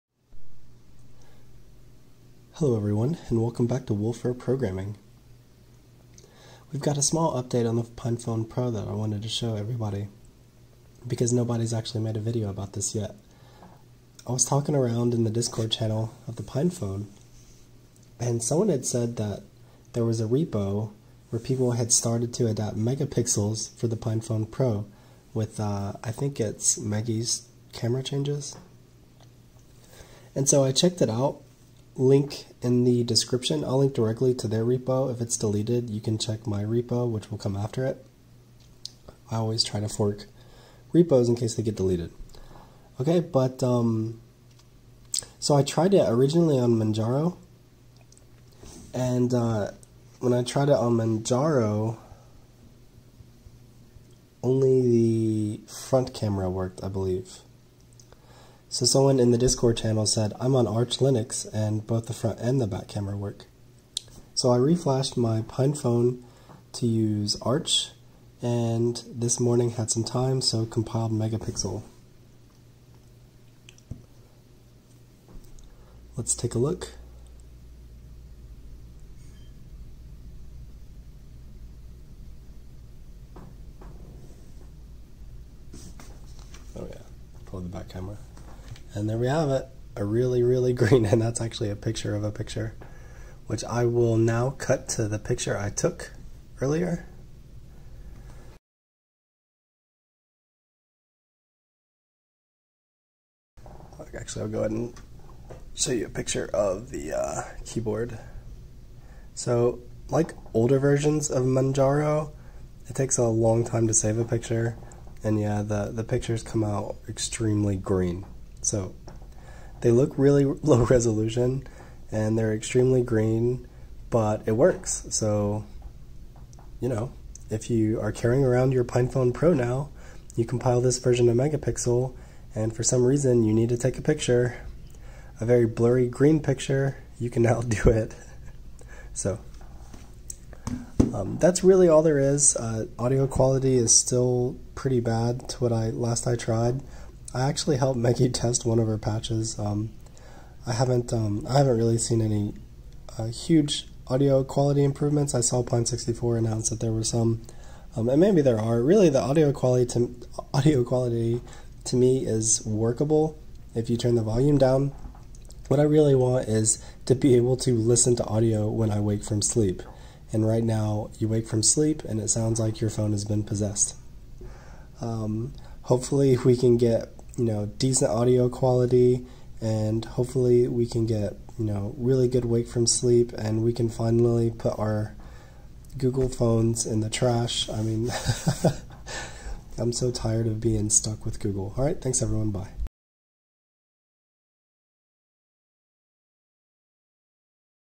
Pinephone Pro Camera working with megapixel
The cameras are working on the latest builds of arch linux. The pictures are green and look very low rez.

Backup encase that one disappears